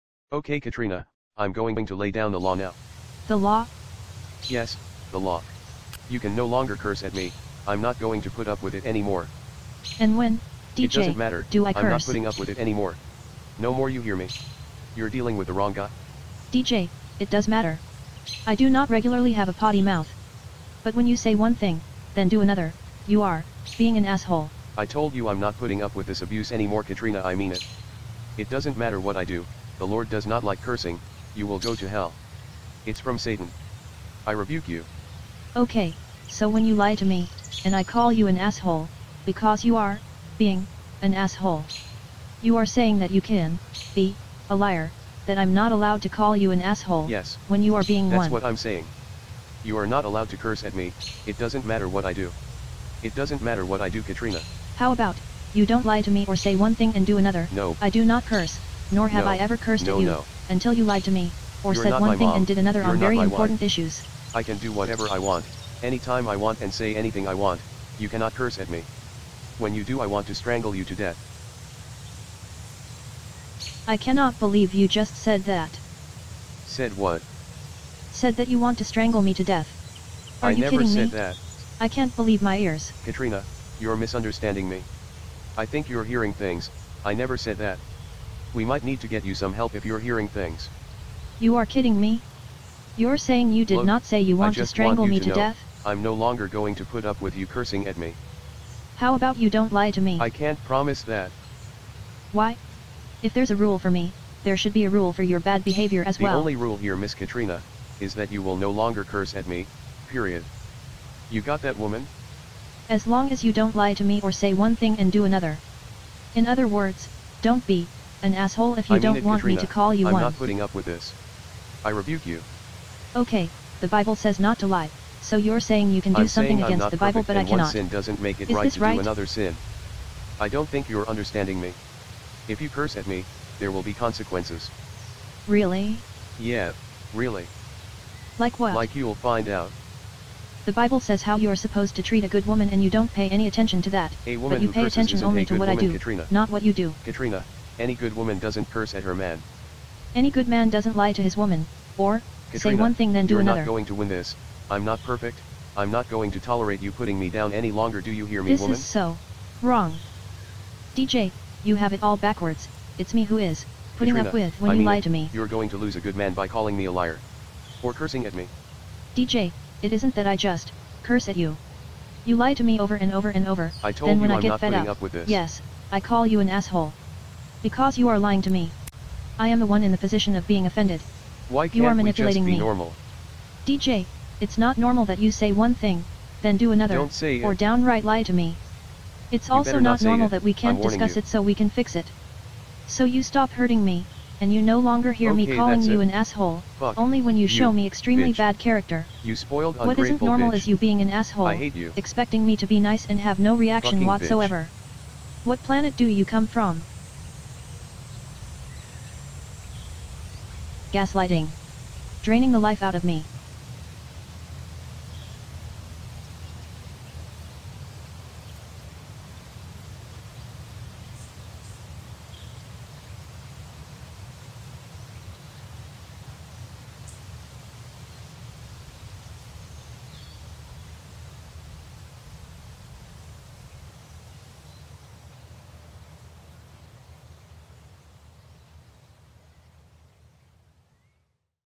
NPD Narcissist Laying Down The Law GASLIGHTING
Yup, this really happened.  I heard this only happens during the "Grand Finale" stage, which this would be & is much different than the first few years of the relationship.  Although the lack of empathy had always been there, I didn't recognize it for a very long time prior to figuring out I was with a narcissist. Double standards, hypocritical rules that do not apply to the narcissist.   When the narcissist hurts you, YOU end up being shamed even though you're in the position of having been offended.